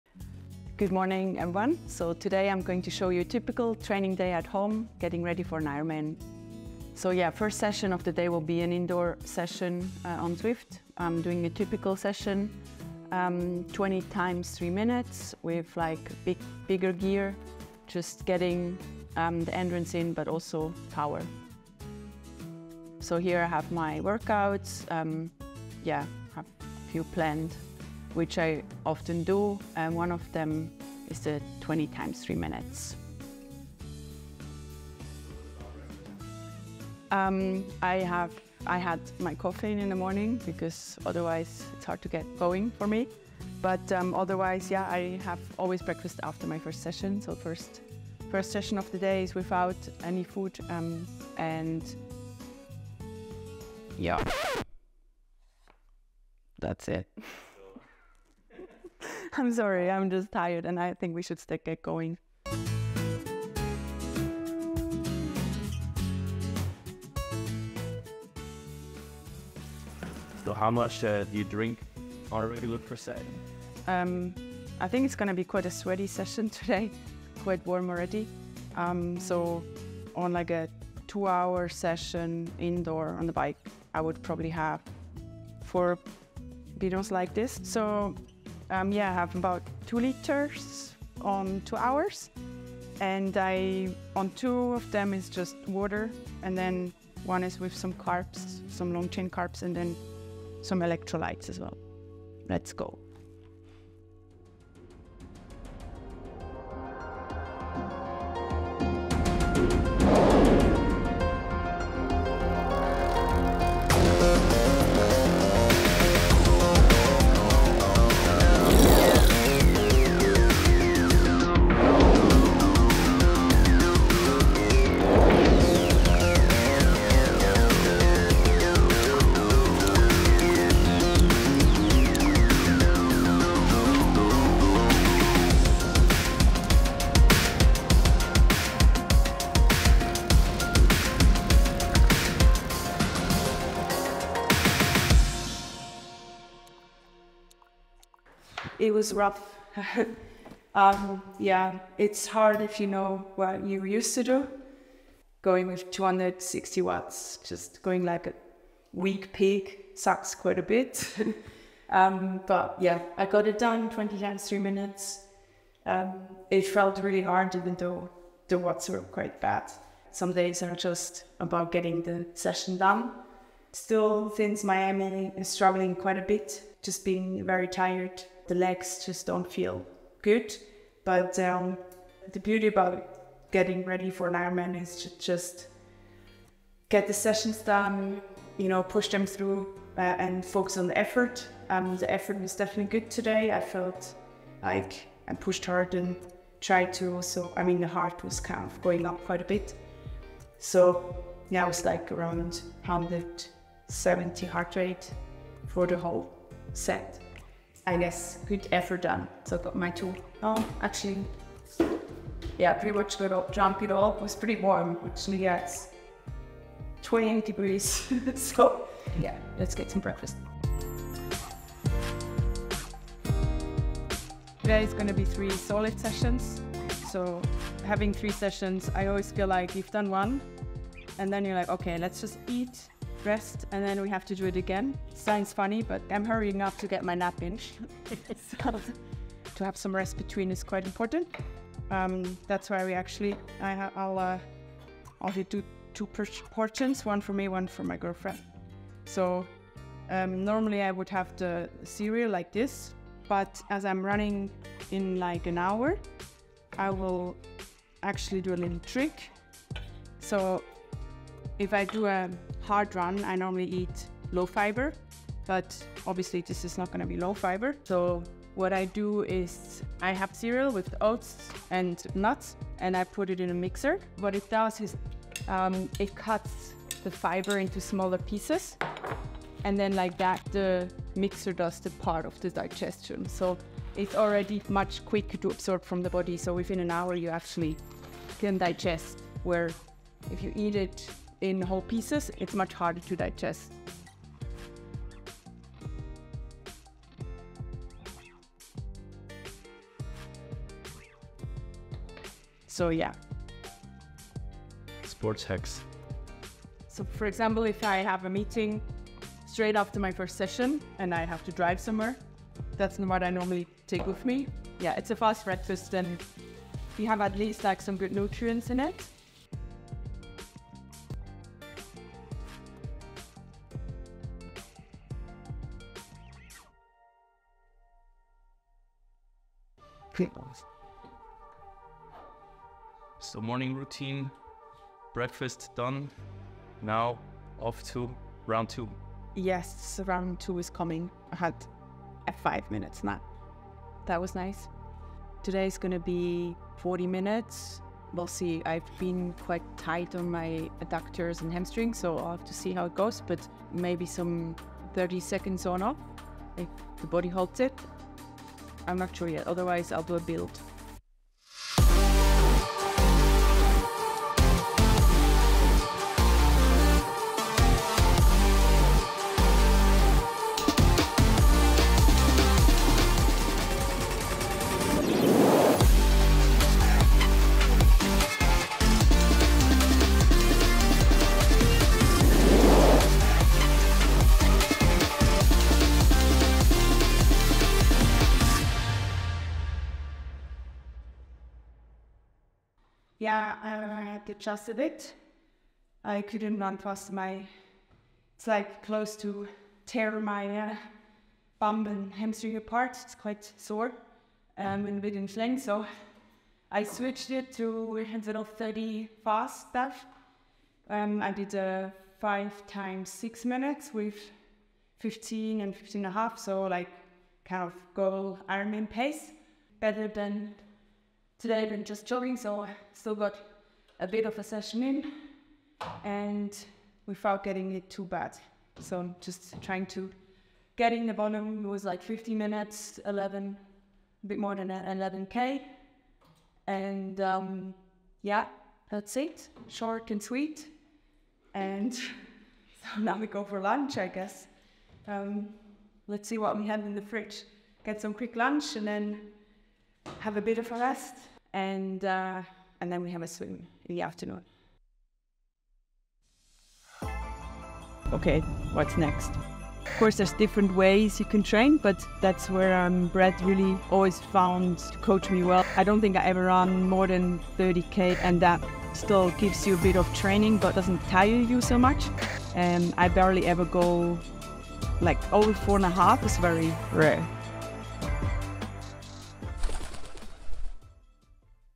How i train for an Ironman indoors: Daniela Ryf's training day (Part 1)
A lot of you are constantly asking me about my training routines, nutrition hacks and much more. Since this is my last season as a professional athlete, I am lifting the curtains and showing you the exact details of how I train for an Ironman. This video shows a typical indoor training day from my home.